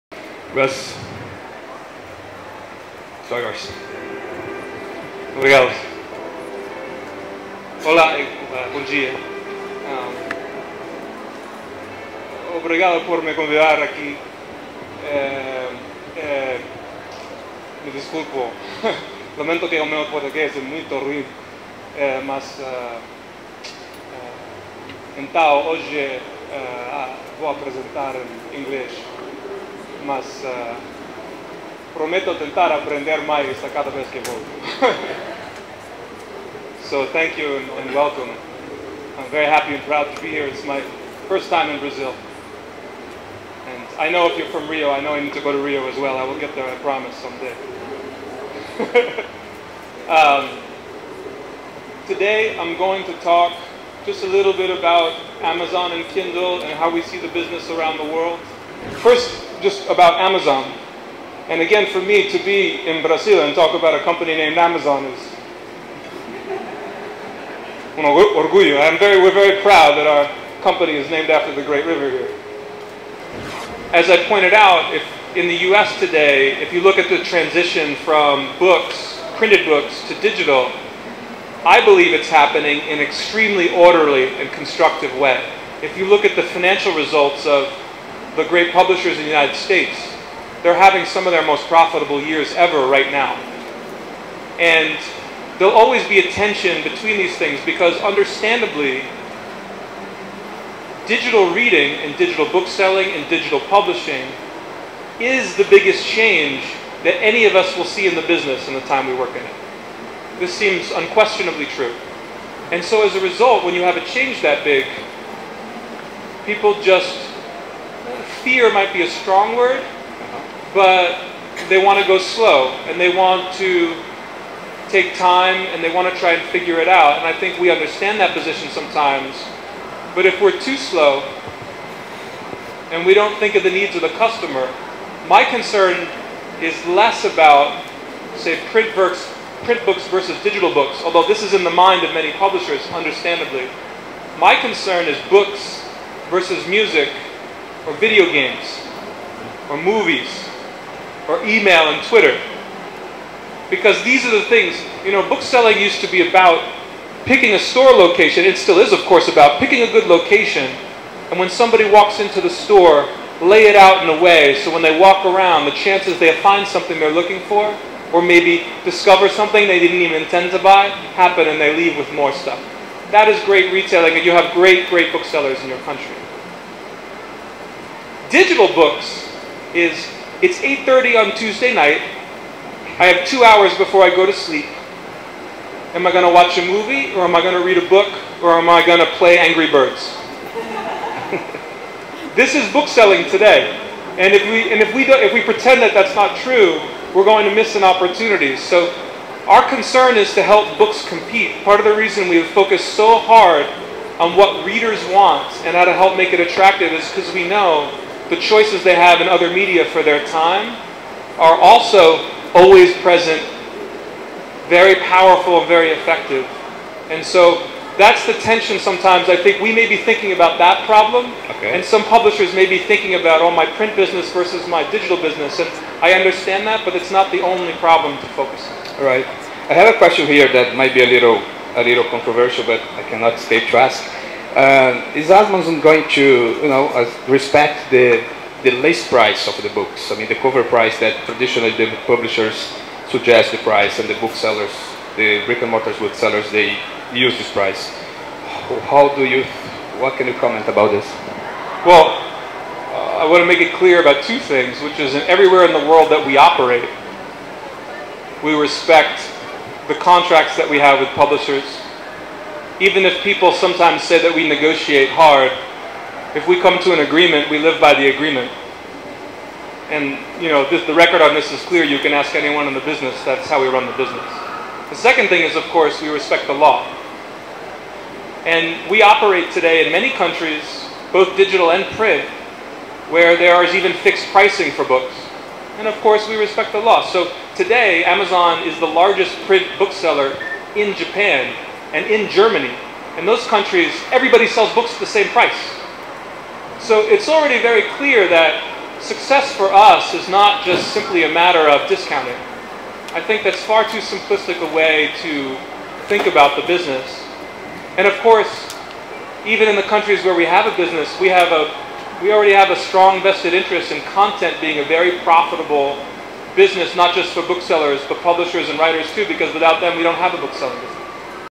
Russ Grandinetti, Amazon's VP, talks about Kindle at the Brazilian Book Fair | part 1/3
Russ Grandinetti, Amazon's vice president who oversees the whole Kindle operation in the world, visited the Brazilian Book Fair (Bienal do Livro de São Paulo) on Aug 10, 2012. He also made an one-hour presentation for Brazilian publishers and talked about the Kindle history and Amazon's mission and mindset behind the development of its digital platform for eBooks. After his lecture, he answered questions from the moderator, Carlo Carrenho, and from the public about DRM, eBook pricing, global markets, the Brazilian book market, educational books, the Kindle price and even reading accessibility for the blind. He also mentioned what he reads on his Kindle and answered to the question "When is Amazon opening on the Moon?"